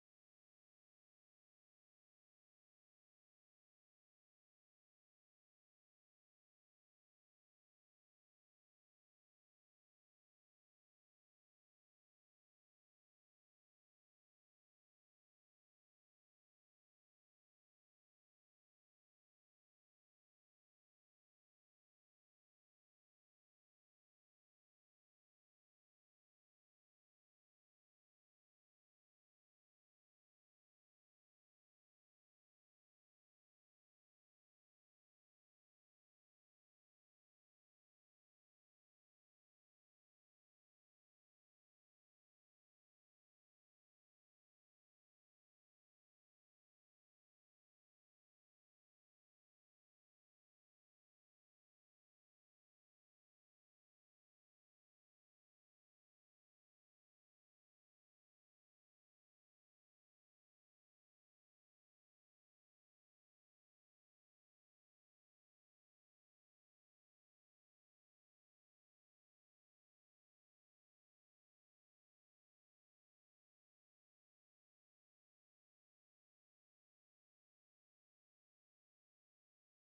Walking Rain
Timelapse of the rain approaching my building. Taken on 28 Dec 2015